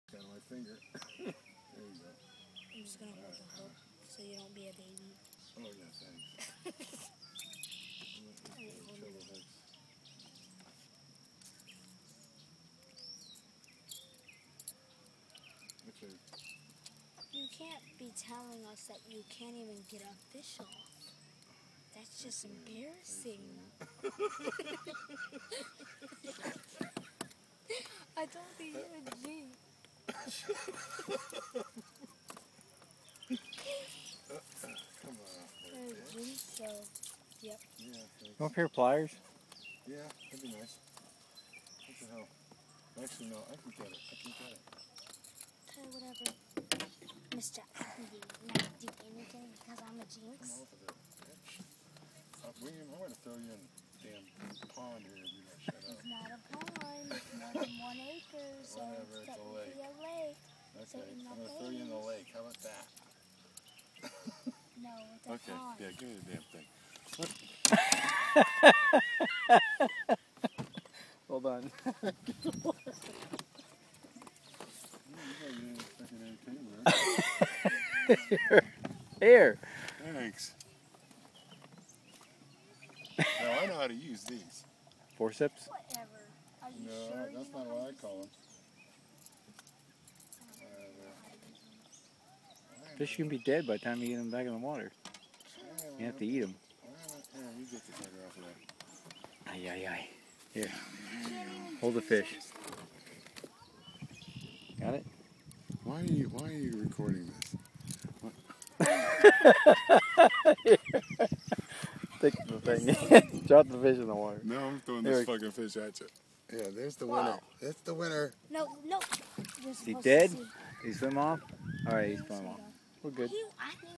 Big bass
Steve Casey catches very big bass but can't get hook out